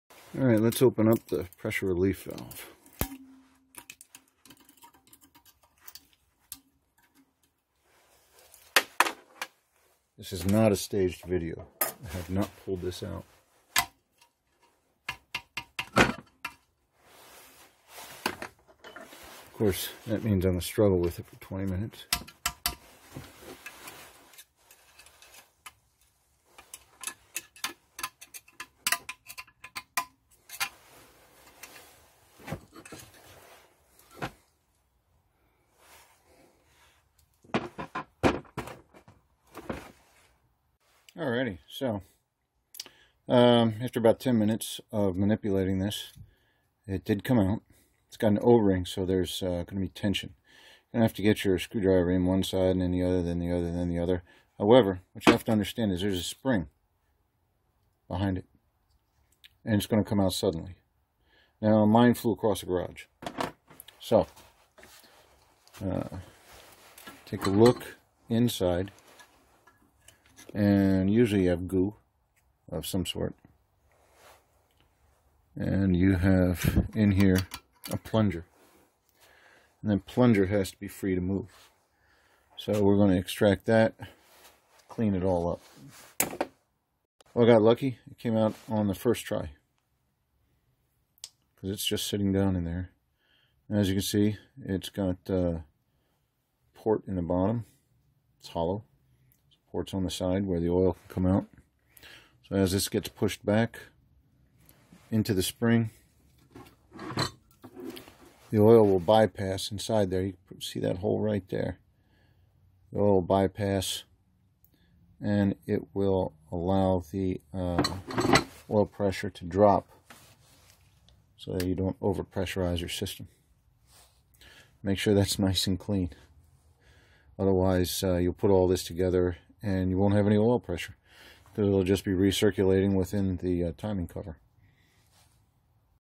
Hidden secret! EASY FIX! Rover 4.0L Oil pressure relief valve removal.
Every engine has an apparatus for regulating the oil pressure.  On a Rover 4.0L it is a serviceable part. If you have the timing cover off then take the opportunity to clean out the valve assembly and install a new o-ring.  If you overheat one of these Rover V8's, then this valve will likely stick open and you won't have any oil pressure.  If your engine takes more than a second to get oil pressure after starting, then clean out this valve.  Dirt and sludge will make it stick open.  The o-ring is a point of leaking that few people think about.  On 99% of the engines on the road, few have a serviceable oil pressure relief valve.  You can also call this a check valve or bypass valve, they are all relevant terms.